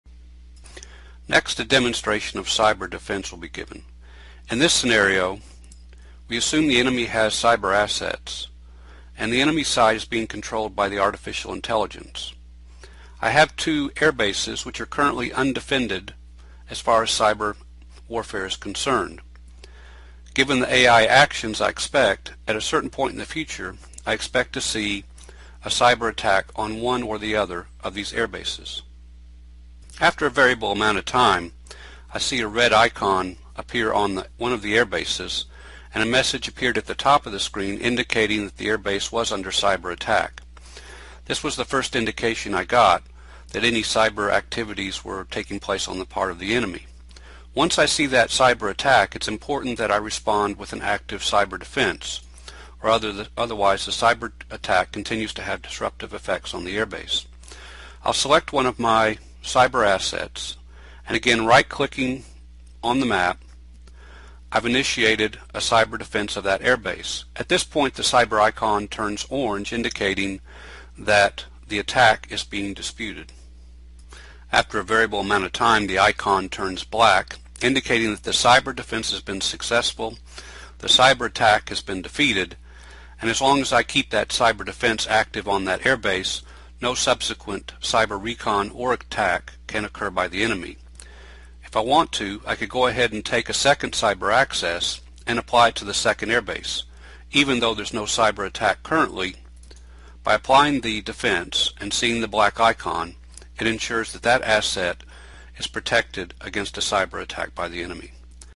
Cyber Defense
This video explains how to deploy a Cyber defense on ADWar using map.exe.